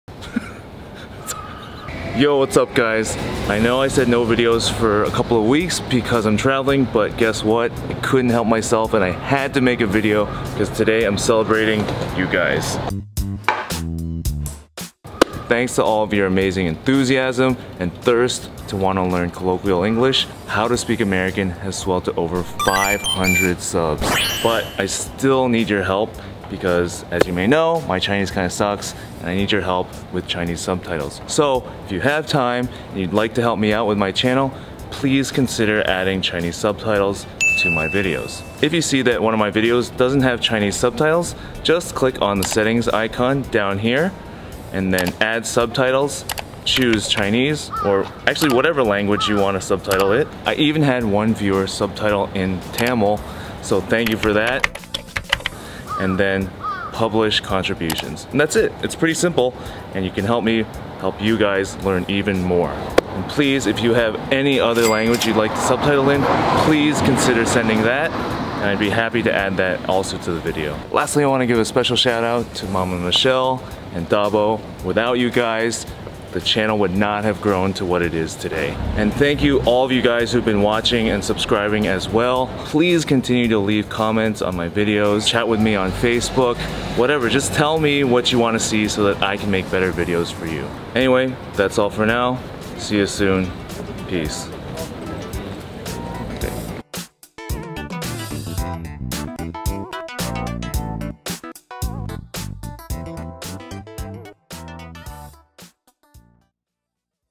Can You Guys Give Me a Hand??? | 請你們幫我一下
I just had to dedicate this one video to all of you gung-ho English learners.  沒有你們這個頻道沒有意義

If you'd like to help me by adding subtitles in Chinese or any other language, follow this link